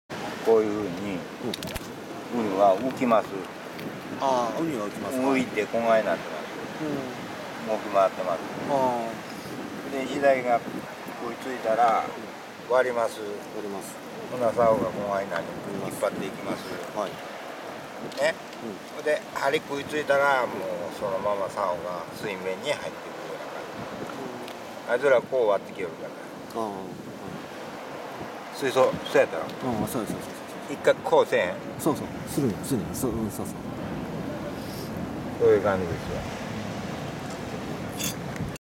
石鯛仕掛の説明
石鯛のウニの食い付き方の説明をベテランSさんに説明してもらっています。